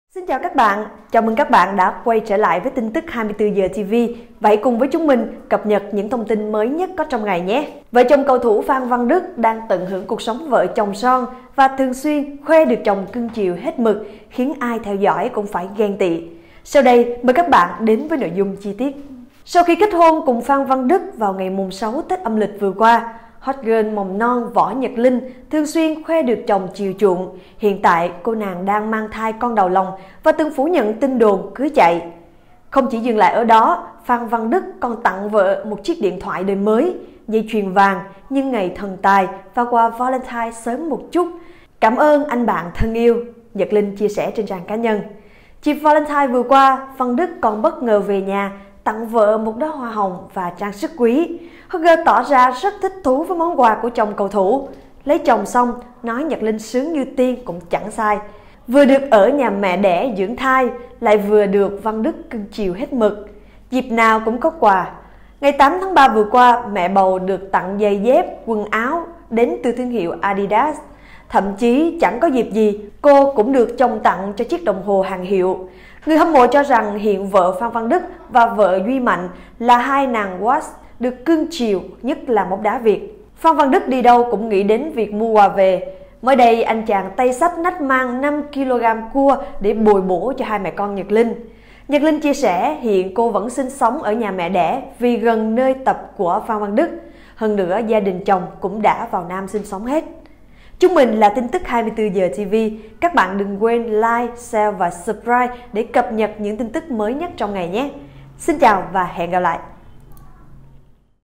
Vợ Cầu Thủ Phan Văn Đức Tiết Lộ Được Chồng Chiều Chuộng Khi Mang Bâ`u - TIN TỨC 24H TV
Vợ Cầu Thủ Phan Văn Đức Tiết Lộ Được Chồng Chiều Chuộng Khi Mang Bâ`u